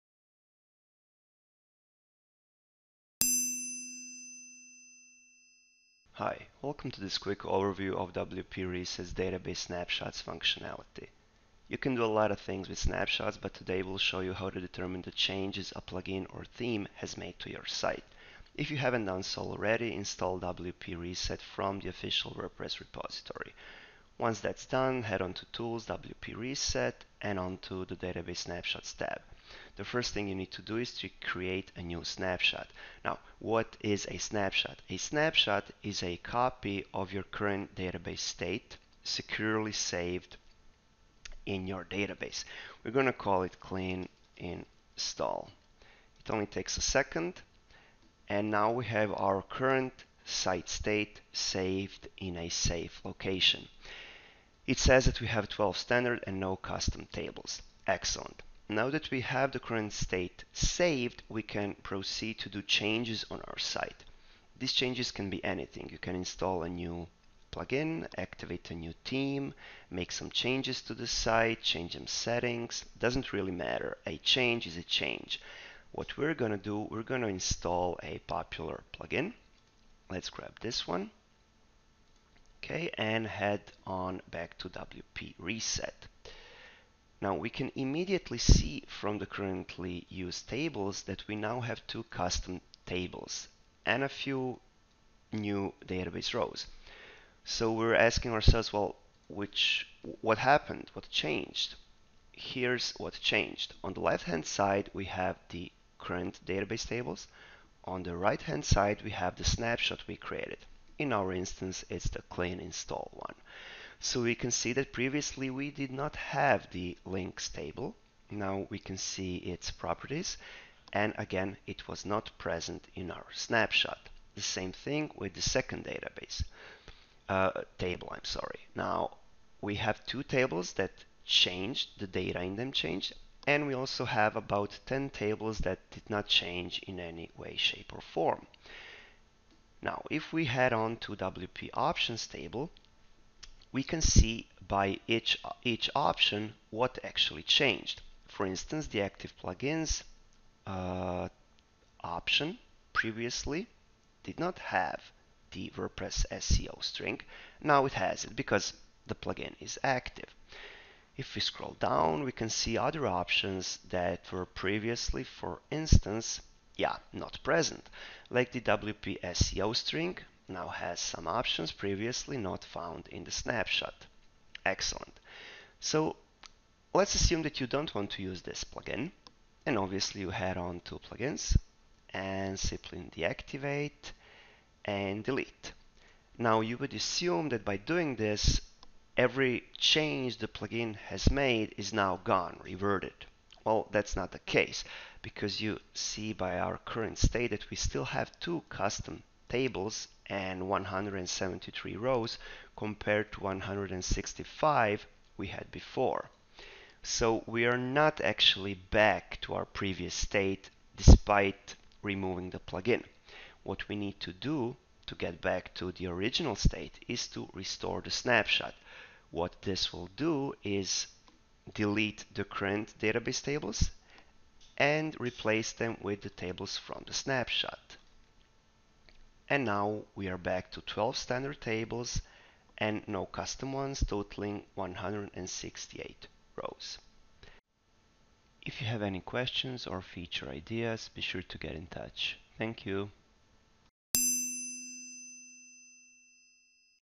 Database Snapshots - WP Reset
WordPress database snapshots ease development by enabling you to quickly create restore points - database snapshots. Those can be restored, compared to the current database or downloaded as gzipped SQL dumps. or view a detailed article about the feature